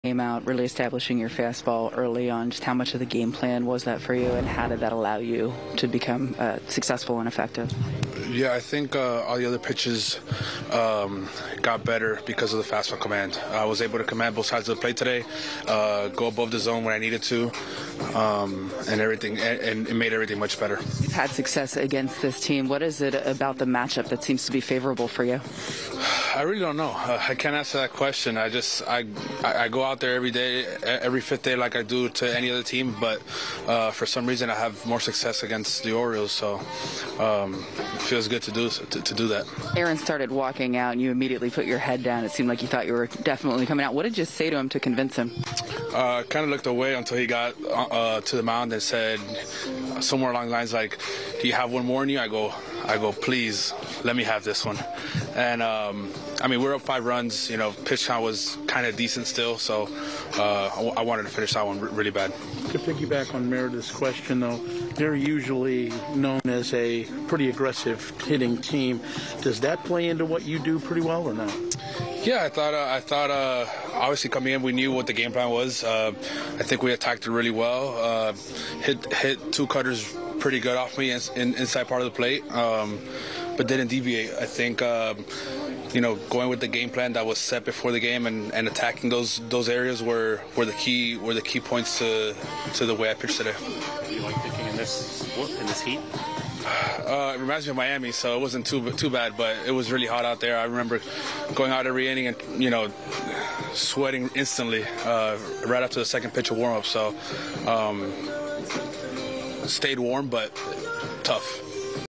Nestor Cortes on his start in the 6-0 victory over Baltimore
Nestor Cortes recaps his performance in his series finale start against the Orioles.